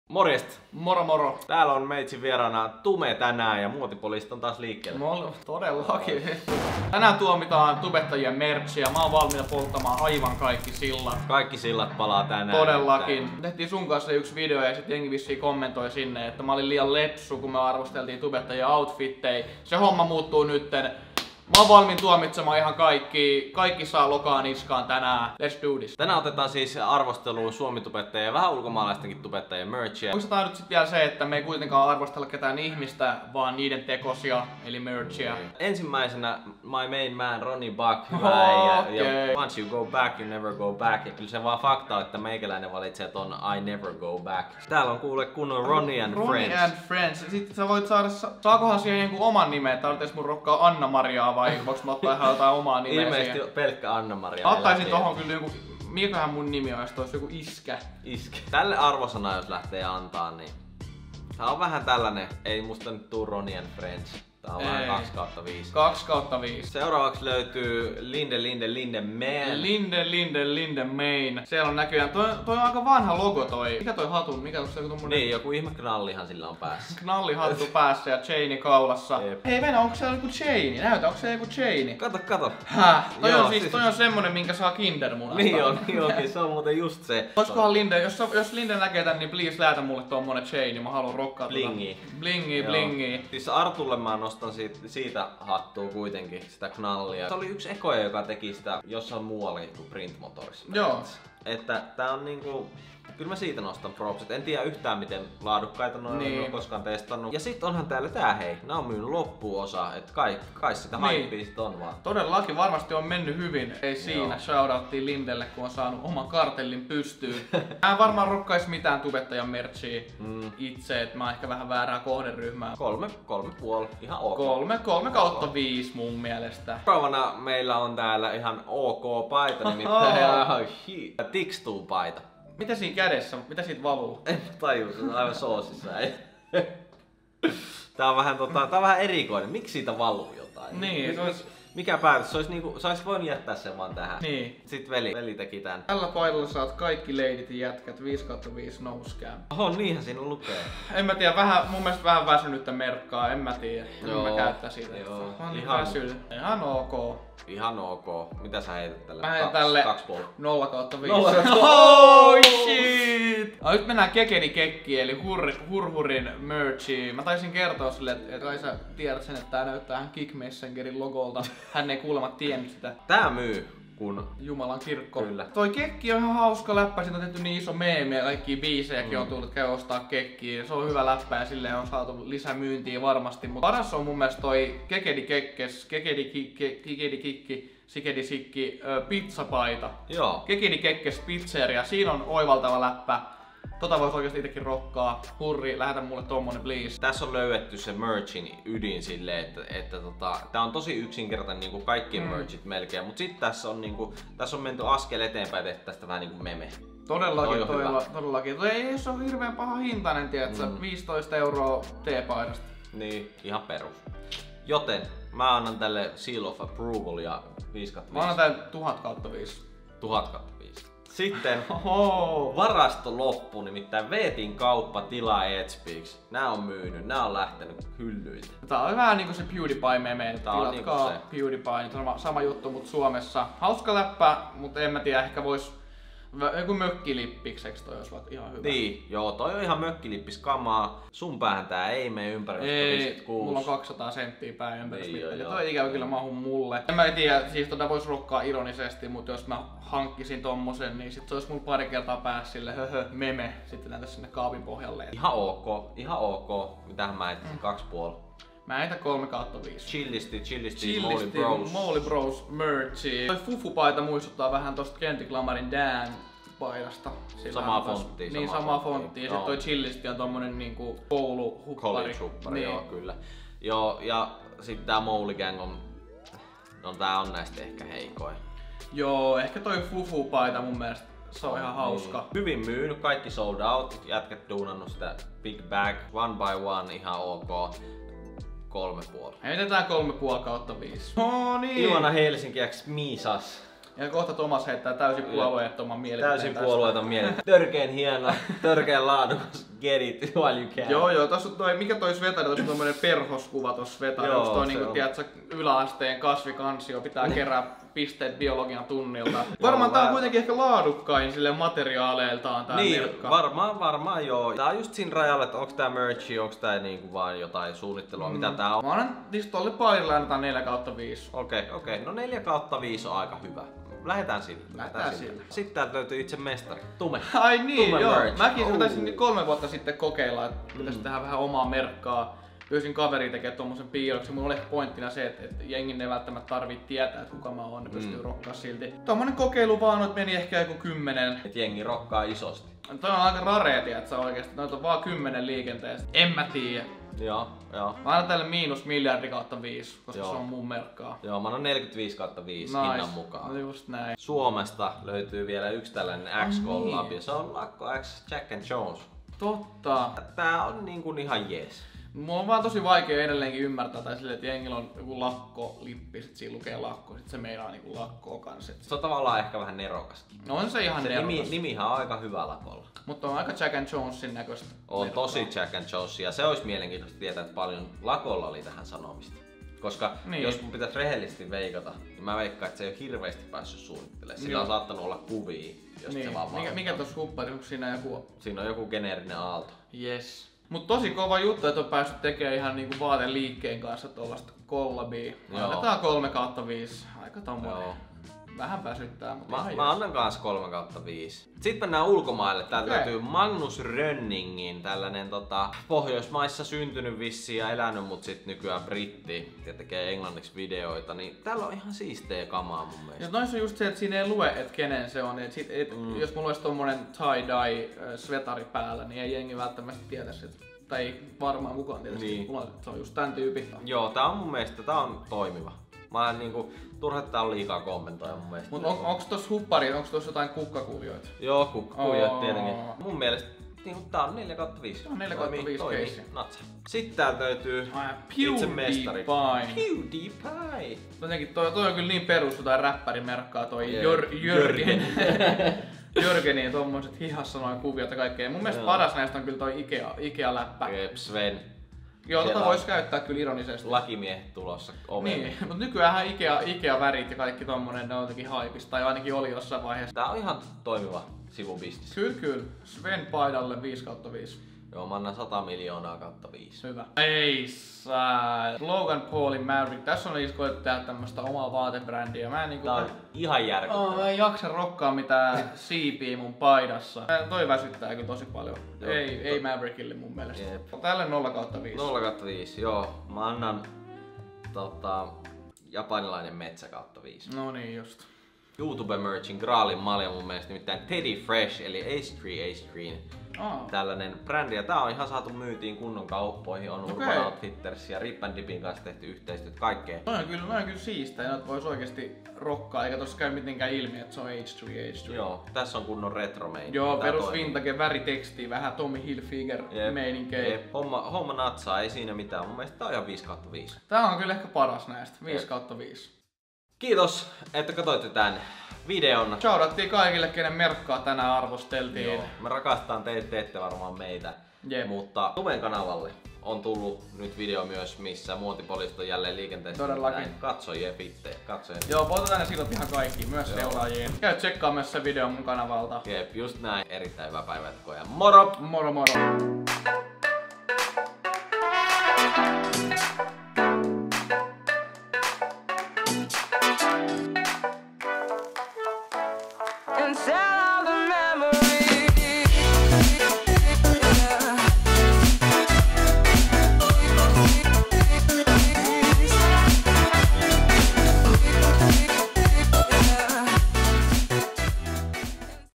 Arvostellaan Tubettajien Merchiä (ft. Tume)
Muotipoliisit on täällä taas! Tällä videolla arvostellaan YouTubettajien merchiä.

TOMAS